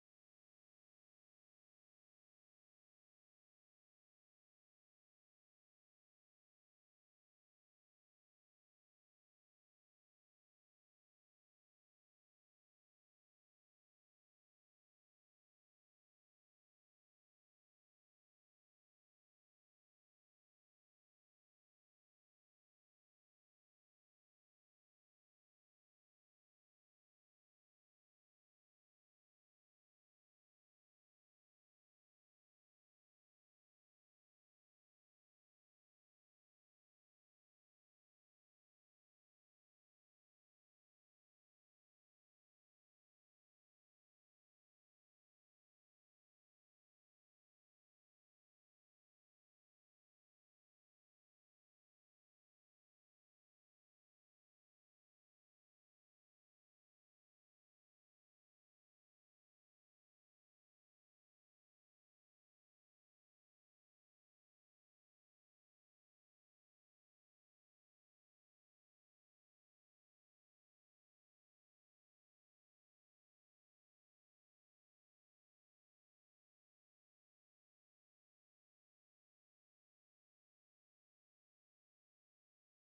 Cloussen's Log Entry 3 - Cockpit Cam Feed
This video contains no audio, tail end of a fight with an NPC Ninetail Gladius.  Recorded for a different project as a component of that project, not truly a video in its own right.